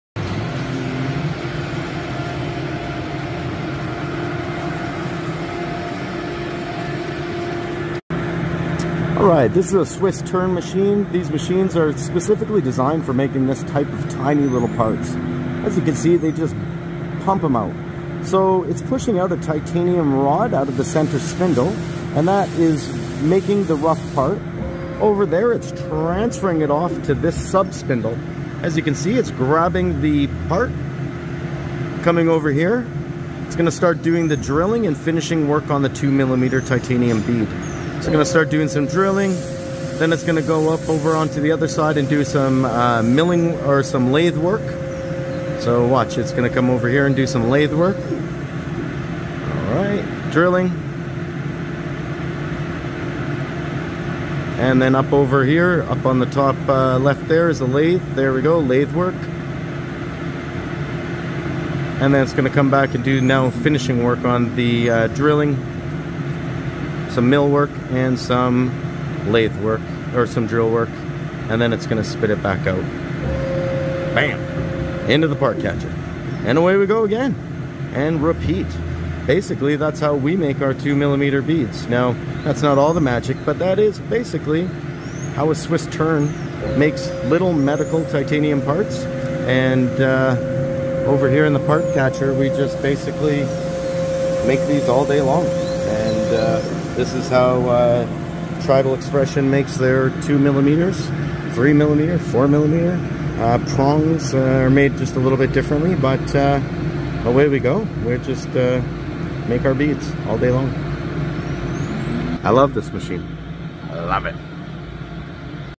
How It’s Made: Titanium Threadless Beads 🇨🇦 | Precision CNC Body Jewelry Manufacturing
Ever wonder how ultra-tiny, high-precision titanium body jewelry is made? Watch as our Tornos Swiss Turn CNC lathe machines 2mm titanium threadless beads from solid ASTM F136 implant-grade titanium—right here in Calgary, Canada.

This is where micron-level accuracy meets artisan-level care. Our in-house manufacturing process ensures consistent fit, biocompatibility, and world-class quality for every piece of jewelry that leaves our facility.

✔️ Medical-grade titanium (ASTM F136)
✔️ Swiss-type CNC lathe machining
✔️ Made 100% in-house in Canada
✔️ Hand-finished for a perfect result
✔️ Built to last. Built to heal.

🎥 Come behind the scenes and see what makes Tribal Expression a global leader in precision body jewelry.